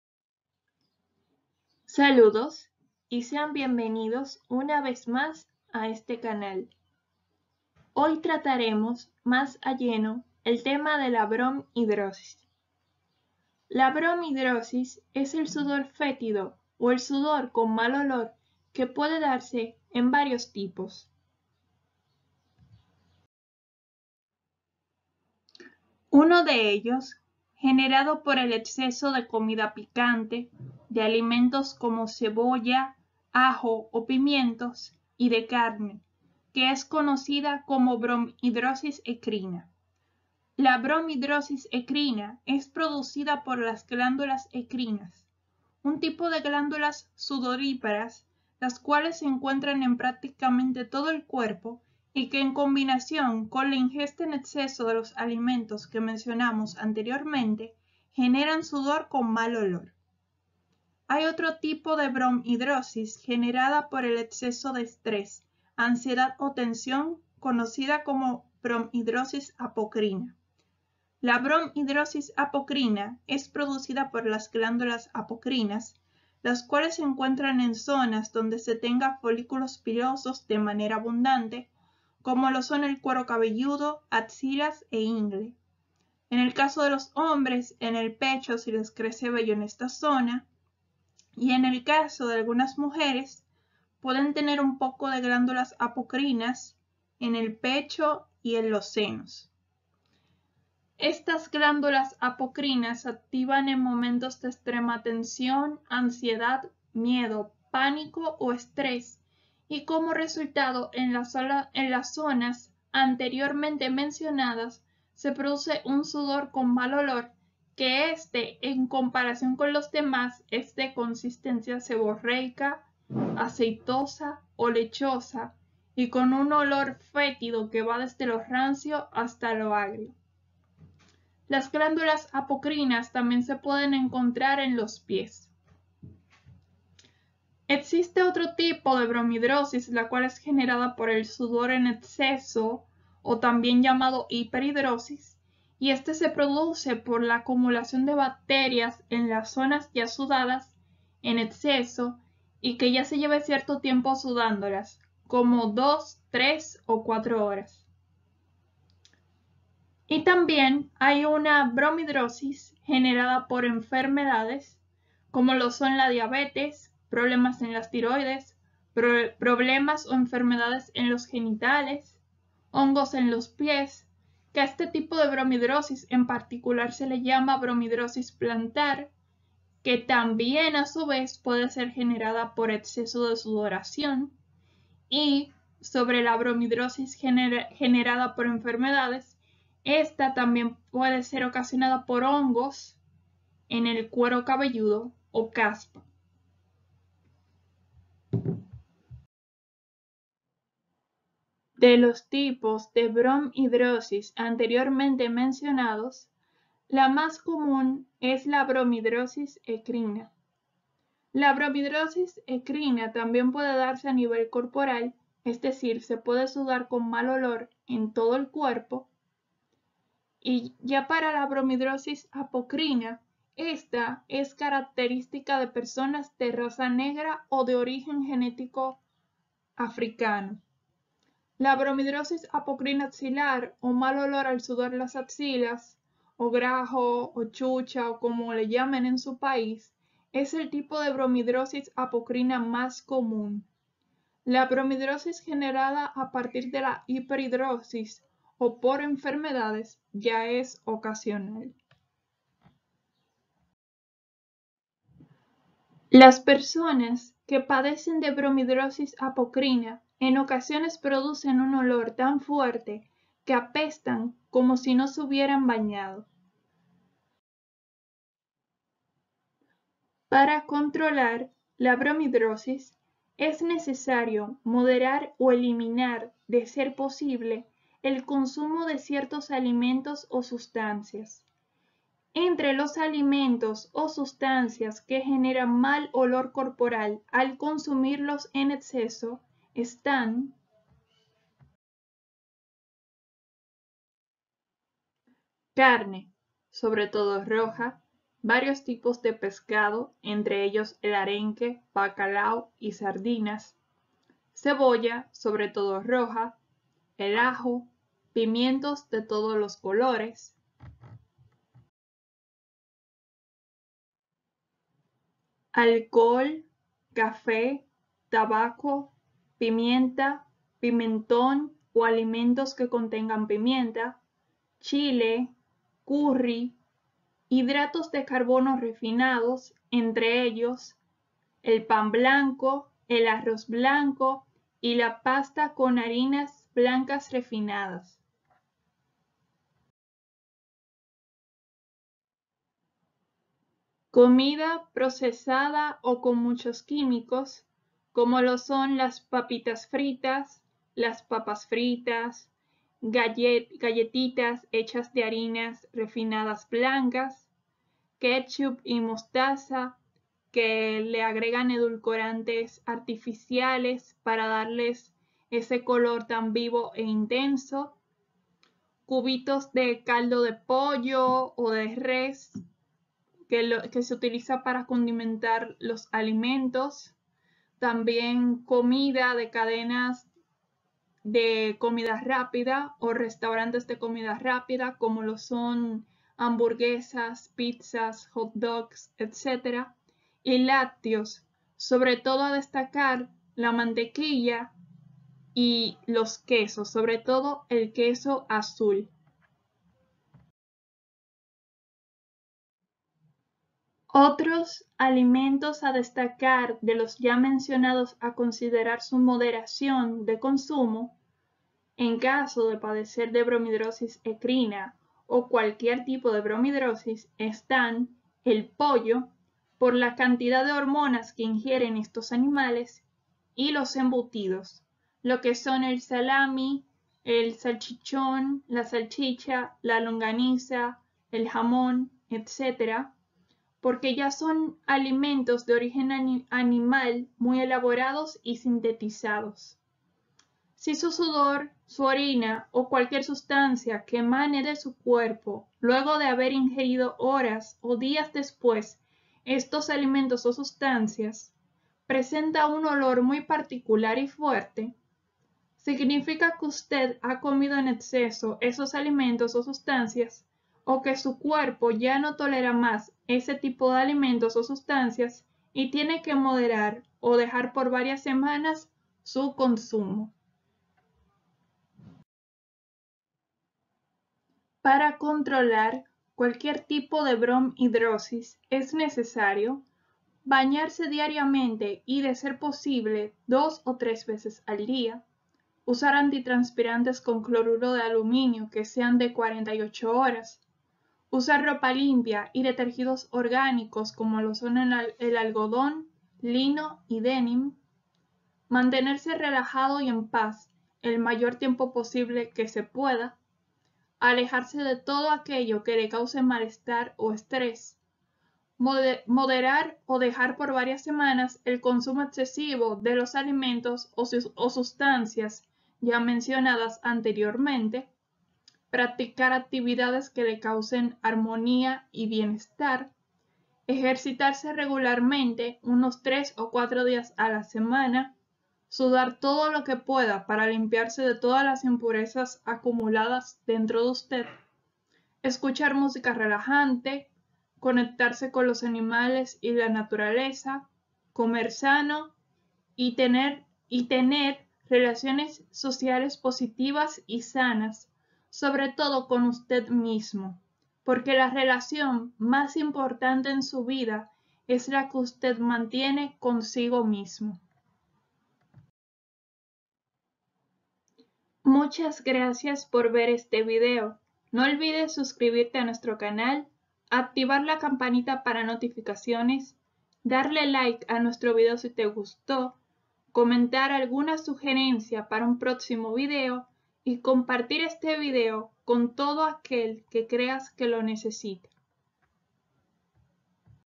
Bromhidrosis [Desglose]
Tratamos mas a profundidad lo que es la bromhidrosis, los diferentes tipos de bromhidrosis existentes y como controlar esta condición metabólica.

Aquí tienes una introducción a lo que es la bromhidrosis y demás condiciones metabólicas ligadas al sudor o al mal olor